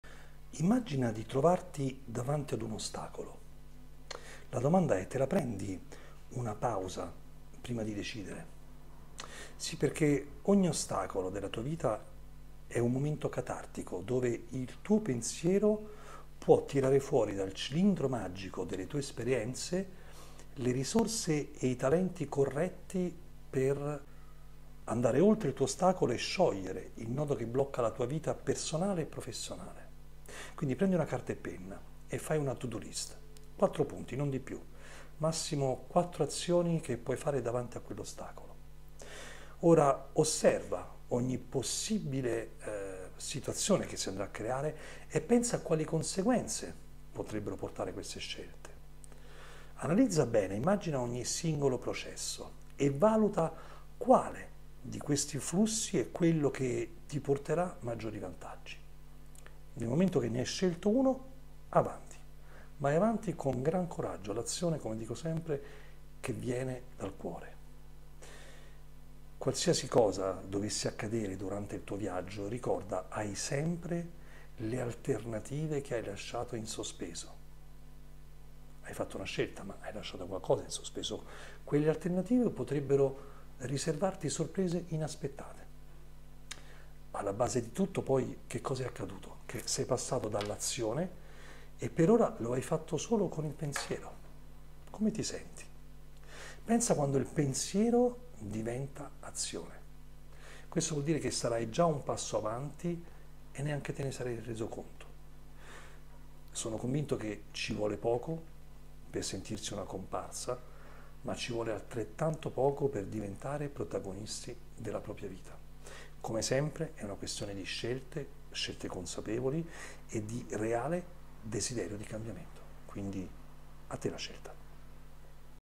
Ostacoli, opportunità di crescita e cambiamento (Pillole di coaching)
Immagina di trovarti davanti ad un ostacolo?
TE LA PRENDI UNA PAUSA PRIMA DI DECIDERE?

Ogni ostacolo é un momento catartico dove il tuo pensiero può tirare fuori dal cilindro magico delle tue esperienze le risorse e i talenti corretta per andare oltre.
Andare oltre il tuo ostacolo e sciogliere il nodo che blocca la tua vita personale e professionale.
Carta e penna con  una
to do list, 4 punti non di più…massimo 4 azioni.

🔭 Ora osserva ogni possibile alternativa e pensa a quali conseguenze potrebbe portare,
🧐 analizza bene, immagina ogni singolo processo,
📈 valuta quale di questi flussi è quello che ti porterà maggiori vantaggi.
💪 E poi avanti tutta con gran coraggio, azione che viene dal cuore. ♥️
Qualsiasi cosa dovesse accadere durante il tuo viaggio ricorda, hai sempre le alternative che hai lasciato in sospeso, potrebbero riservare sorprese inaspettate.
Alla base di tutto cosa è accaduto?
Sei passato all’azione e per ora lo hai fatto solo con il pensiero, come ti senti?
Pensa quando il pensiero diventa azione!
Sarai già ad un passo avanti e neanche te ne sarai reso conto.
Ci vuole poco per sentirsi una comparsa ma ci vuole altrettanto poco per Diventare protagonisti della propria  vita, come sempre una questione di scelte consapevoli e reale desiderio di cambiamento